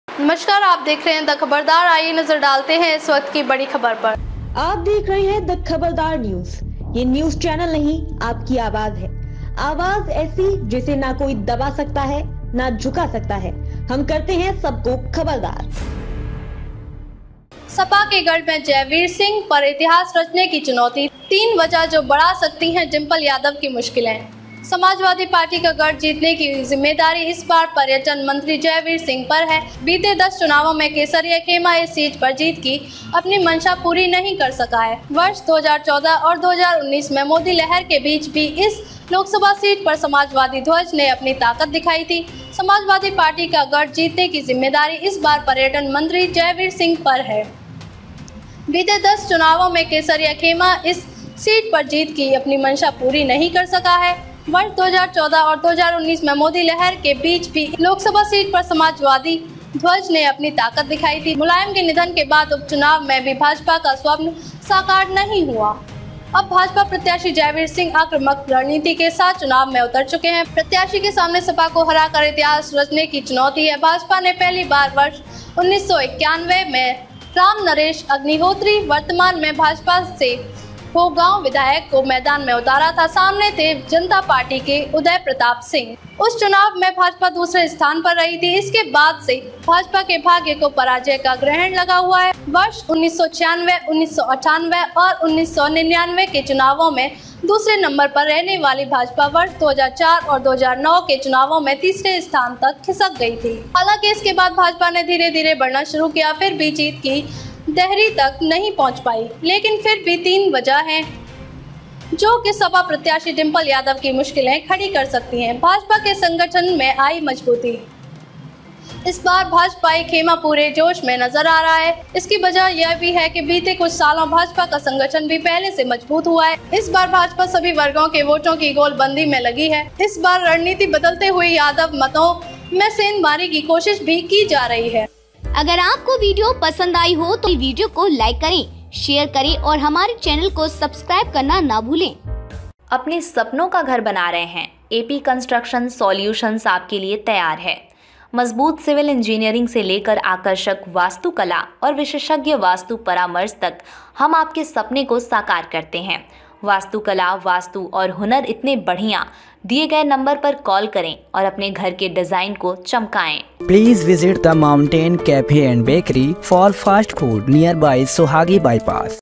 SP के गढ़ मे Jaiveer Singh पर इतिहास रचने की चुनौती तीन वजह जो बढ़ा सकती है Dimple Yadav की मुश्किले
समाजवादी पार्टी का गढ़ जीतने की जिम्मेदारी इस बार पर्यटन मंत्री जयवीर सिंह पर है। बीते 10 चुनावों में केसरिया खेमा इस सीट पर जीत की अपनी मंशा पूरी नहीं कर सका है। वर्ष 2014 और 2019 में मोदी लहर के बीच भी इस लोकसभा सीट पर समाजवादी ध्वज ने अपनी ताकत दिखाई थी